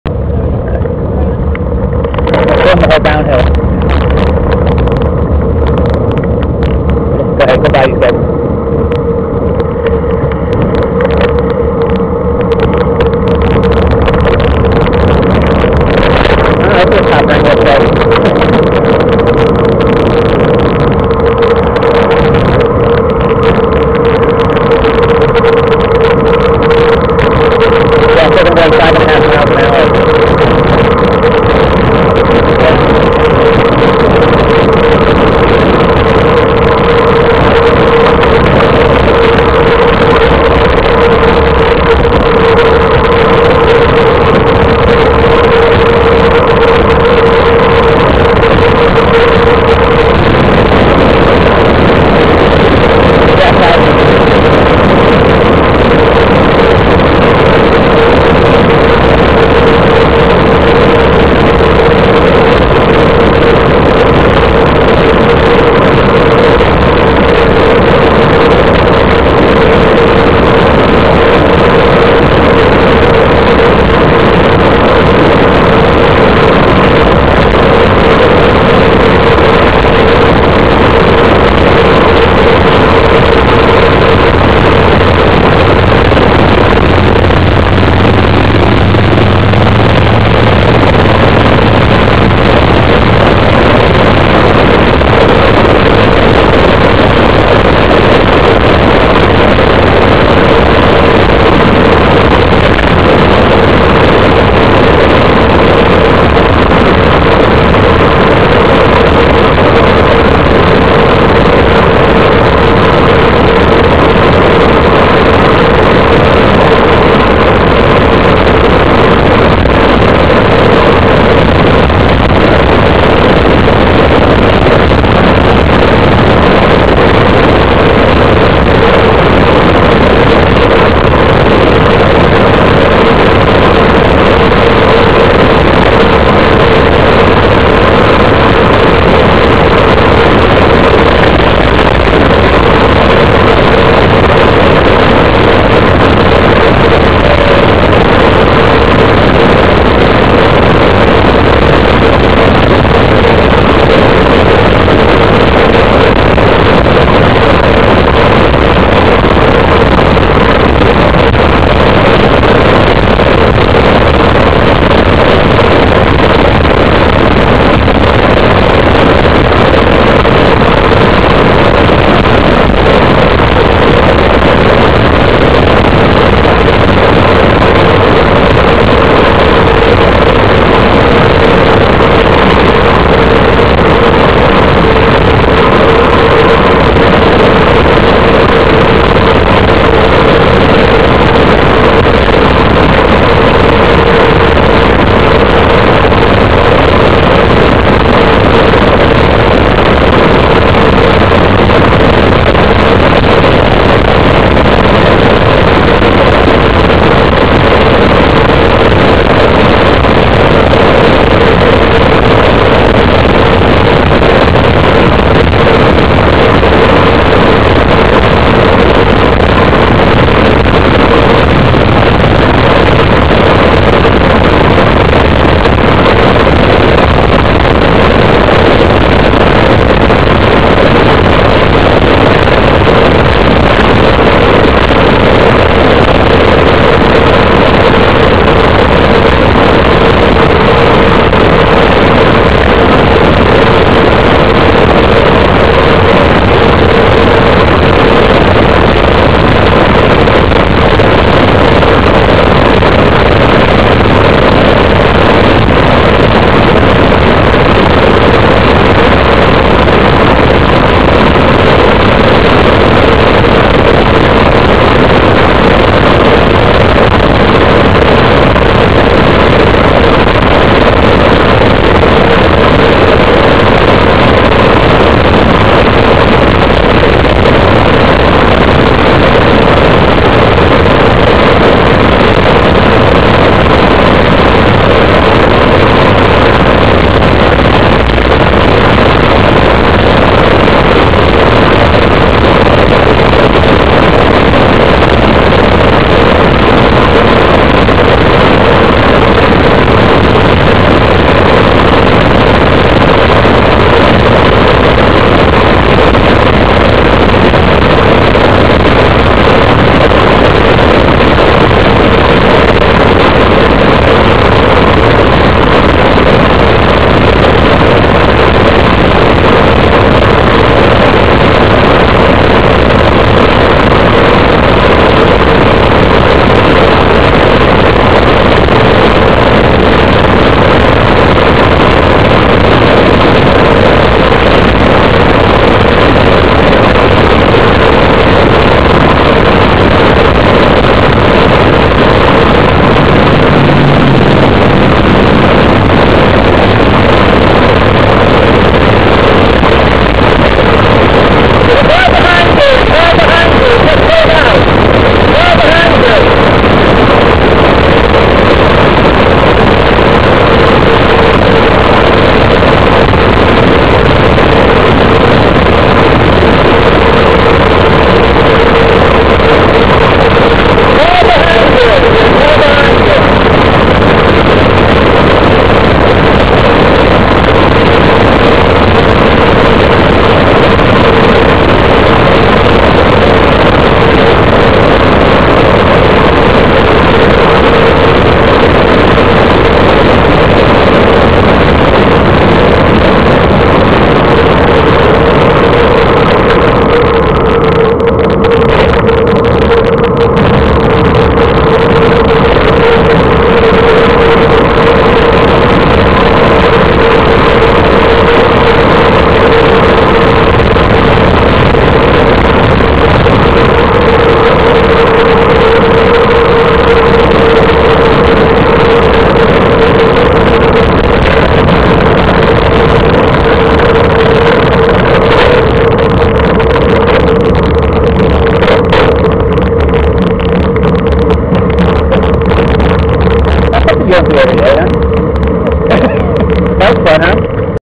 2008 3 Gap 50 Neels Gap Descent
Descent from Neels Gap in the N. Georgia Mountains during the 2008 3 Gap 50 Cycling Ride.